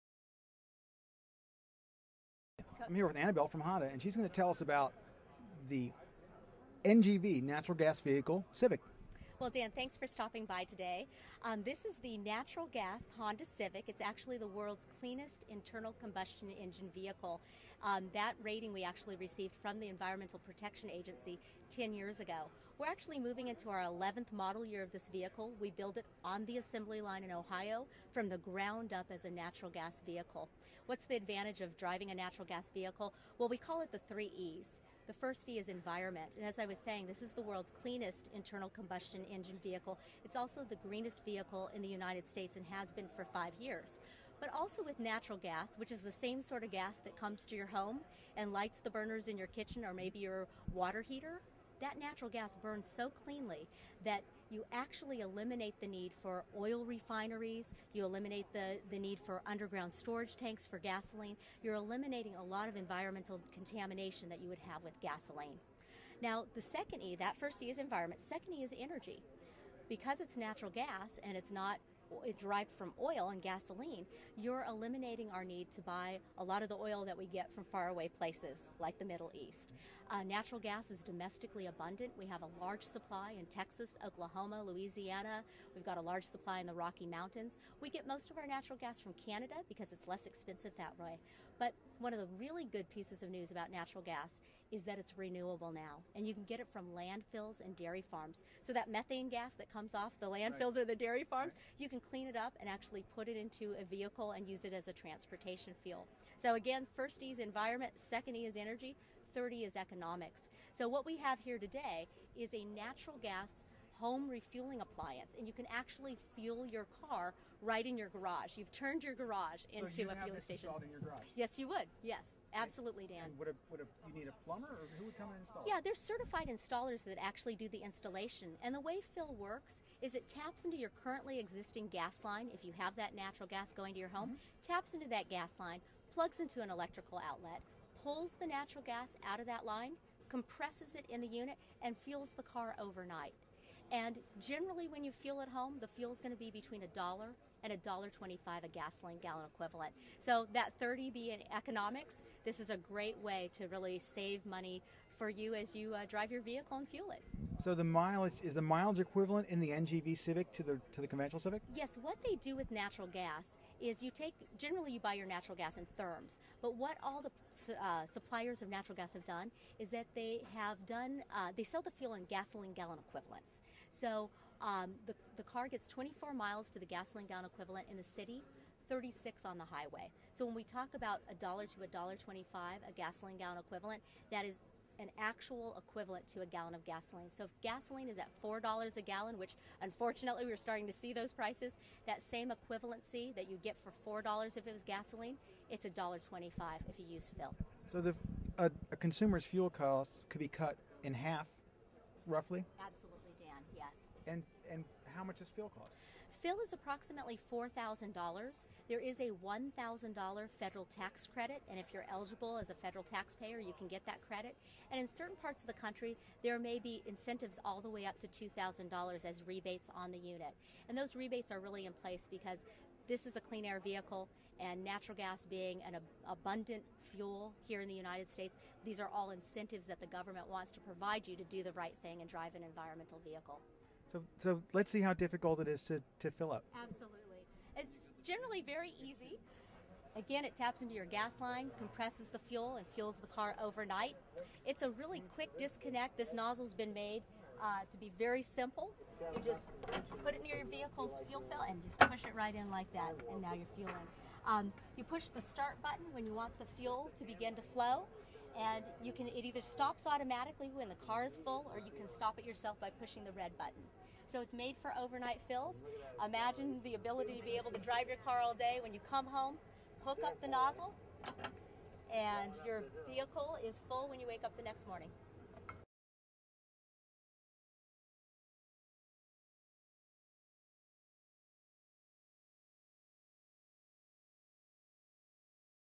Honda Civic NGV & Phill
MPGomatic.com checks out the clean and thrifty Honda Civic NGV (natural gas vehicle) and Phill home refueling station at the '08 Alternative Fuels and Vehicles conference.